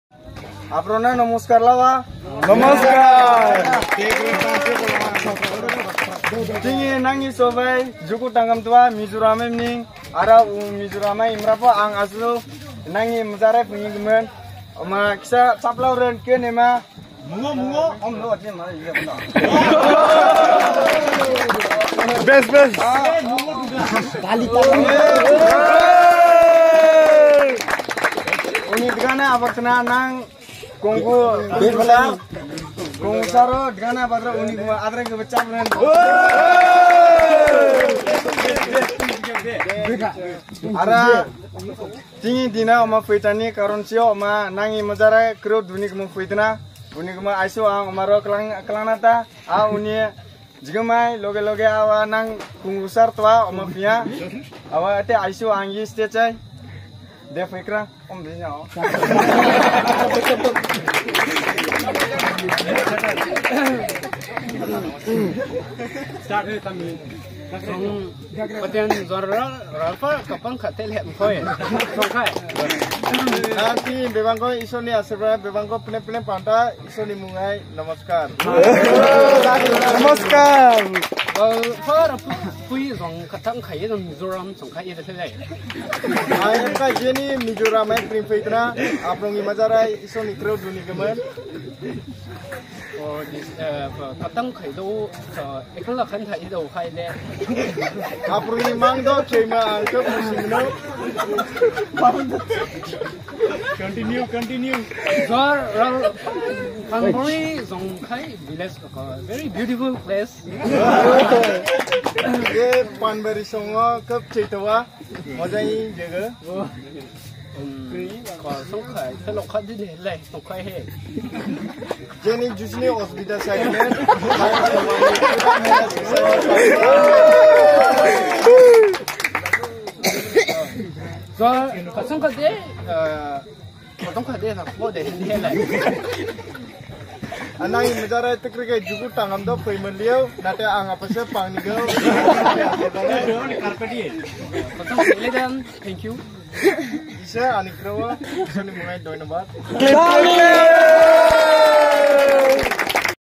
Panbari Soba 2022(Special night)//Special chief guest//Special moment.
when I went to Panbari conference,I had fun with some boys we just make a short video for entertainment.

Your Quaris-
creatersearch:Panbari Soba (Special night)//Special chief guest//2022